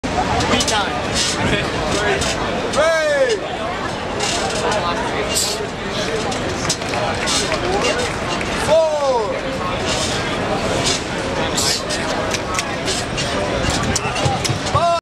Marine does college-boy flip style pull-ups in Times Square
NEW YORK -- Sgt. Robert Stull, Marine Corps Martial Arts Program instructor trainer, 24th Marine Expeditionary Unit, college boy rolls on a pull up bar during Marine Day Times Square, May 27. The Seminole, Fla., native is one of more than 3,000 Marines, Sailors and Coast Guardsmen in the area participating in community outreach events and equipment demonstrations as part of Fleet Week New York 2011. This is the 27th year New York has hosted the sea services for Fleet Week. (Official Marine Corps photo by Sgt. Randall A. Clinton / RELEASED)